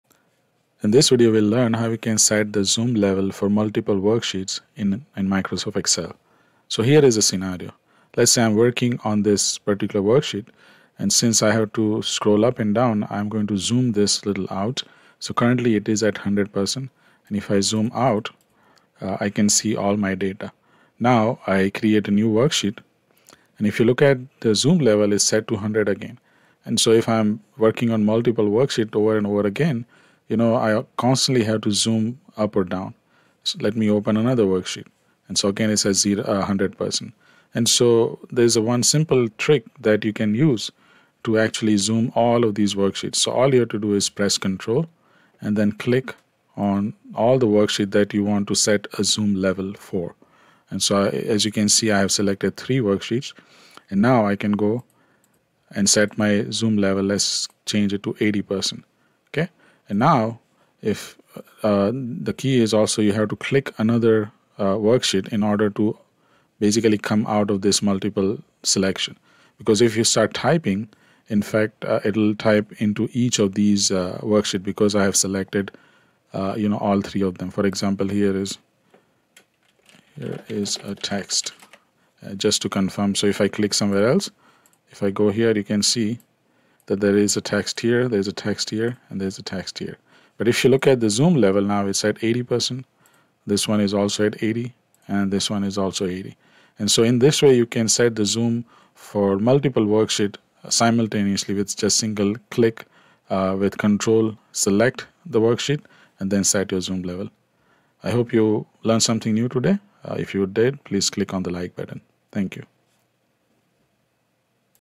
Excel Tip - Zoom Multiple Worksheets with Single Click
This Excel tip shows you how to set zoom level of multiple worksheets in a Workbook with a single click. All you have to do is to press Control and click on all the worksheets that you want to zoom to certain level. Make sure to click on other worksheets to unselect the sheets.